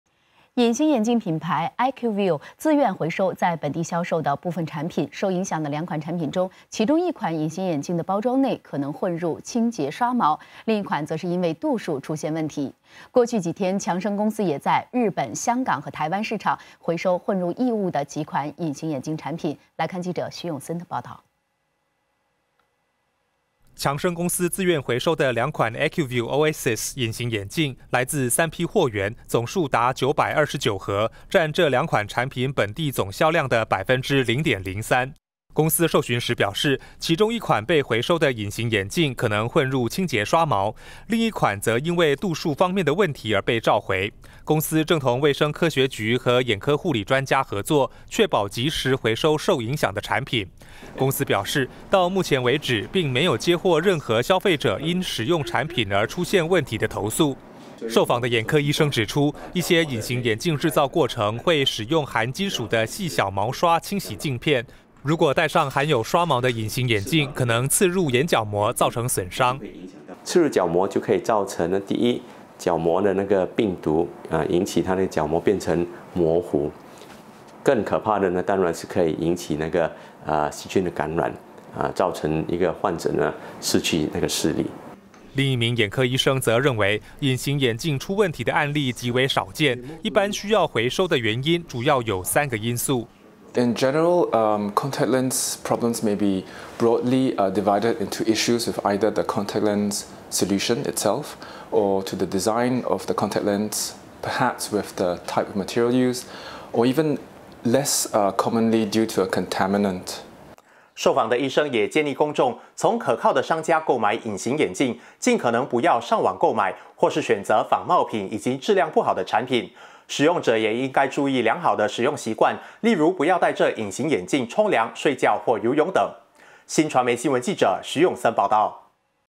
强生公司：自愿回收 两款隐形眼镜三批货源
隐形眼镜品牌Acuvue自愿回收在本地销售的部分产品。受影响的两款产品中，其中一款隐形眼镜的包装内，可能混入清洁刷毛，另一款则是因为度数出现问题。过去几天，强生公司也在日本，香港和台湾市场回收混入异物的几款隐形眼镜产品。